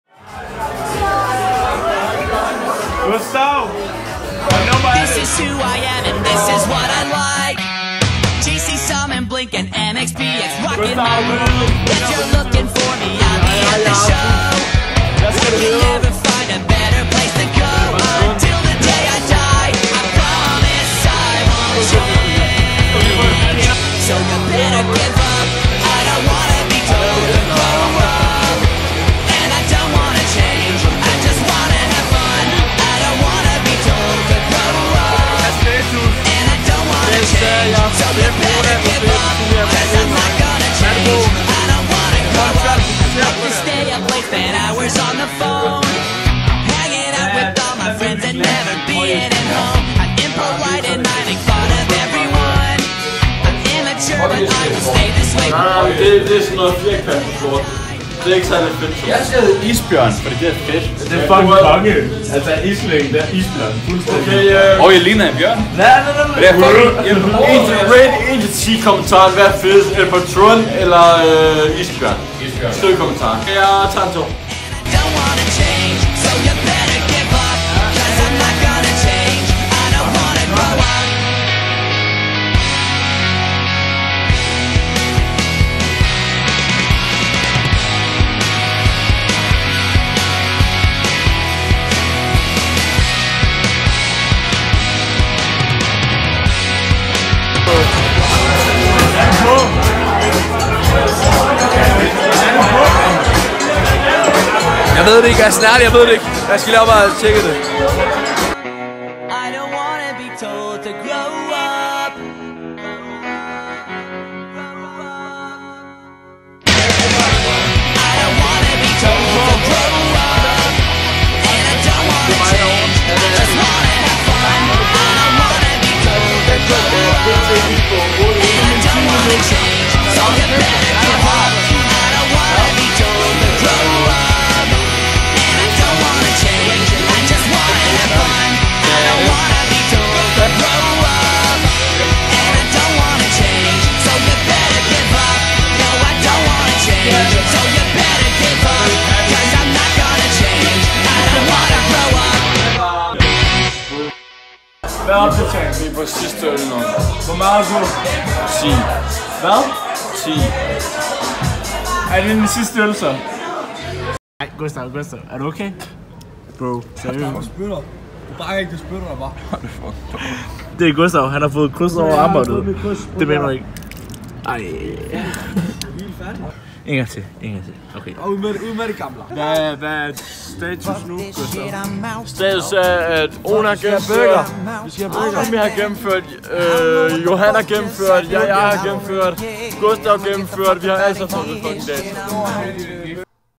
Dato Med Gutterne På Viggos 7/4 2018
Jeg var en tur med drengene inde på Viggos i Odense, for at drikke min første dato. Det er altid hyggeligt på Viggos og især med godt selskab. Så drikker man 10 fadøl hurtigt!